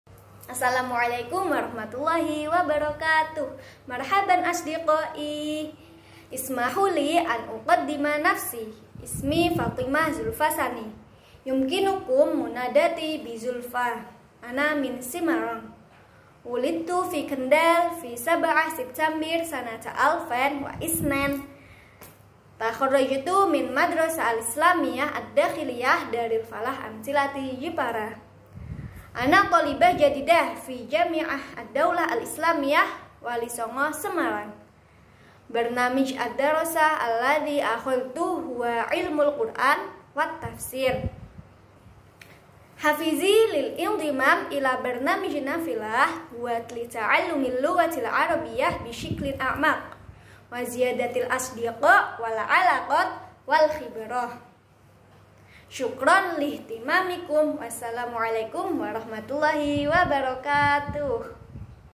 Tes Lisan UKM Nafilah UIN Walisongo Semarang
السلام عليكم و رحمة الله وبركاته

مرحبا أصدقائي
Halo teman-teman

إسمحوا لي أن أقدم نفسي
Izinkan saya memperkenalkan diri saya

إسمي فاطمة زلفى ثاني
Nama saya Fatimah Zulfa Tsani

يمكنكم مناداتي بزلفى
Kalian bisa memanggil saya Zulfa

انا من سيمارانج
Saya dari Semarang

ولدت في كندال في سبعة سبتمبر سنة ألفين وإثنين
Saya lahir di Kendal, tanggal 7 September 2002

تخرجت من مدرسة الإسلامية الداخلية دار الفلاح أمثلتي جبارا
Saya lulusan dari pondok pesantren Darul Falah Amtsilati Jepara

أنا طالبة جديدة في جامعة الدولة الإسلامية واليسونغو سيمارانج
Saya mahasiswa baru di UIN Walisongo Semarang

برنامج الدراسة الذي أخذته هو علم القرآن والتفسير
Prodi yang saya ambil yaitu Ilmu Al-Qur'an dan Tafsir (IAT)

حافزي للانضمام إلى برنامج نافلة هو لتعلم اللغة العربية بشكل أعمق و زيادة الأصدقاء والعلاقات والخبرة
Motivasi saya mengikuti UKM Nafilah adalah untuk belajar bahasa Arab lebih dalam lagi, menambah teman, relasi, juga pengalaman

شُكْرًا عَلَى اِهْتِمَامِكُمْ
Terimakasih atas perhatiannya

وّالسَّلَامُ عَلَيْكُمْ وَرَحْمَةُ اللّه وَبَرَكَاتُهُ.